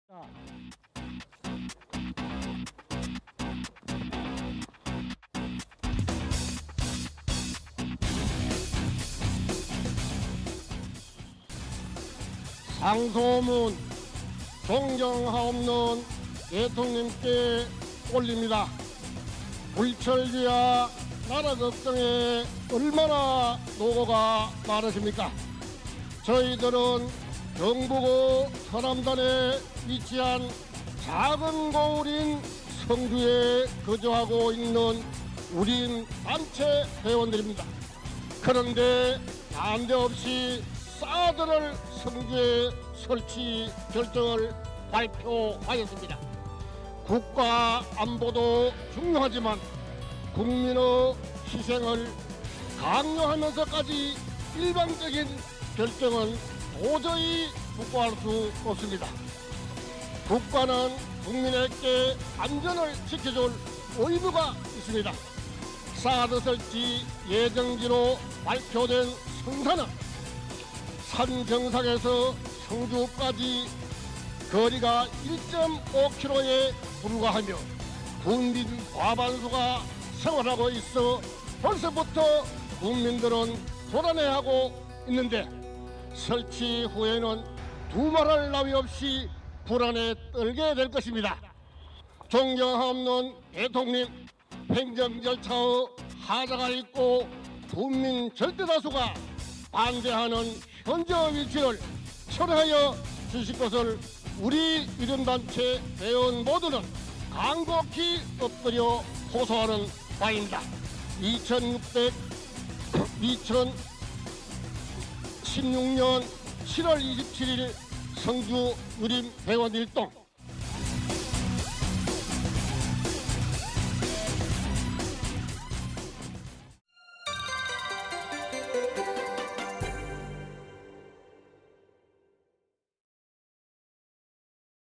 [상소문 요약] 사드 배치, 통촉하여 주시옵소서
27일 오전 서울 종로 청운효자동 주민자치센터앞에서 성주군 유림단체 사드배치 반대 기자회견이 열렸다.
이 영상은 유림단체의 상소문을 요약했다.

(구성 : 황지희 기자 / 촬영 : 정현덕 기자 / 편집 : 박소영 기자)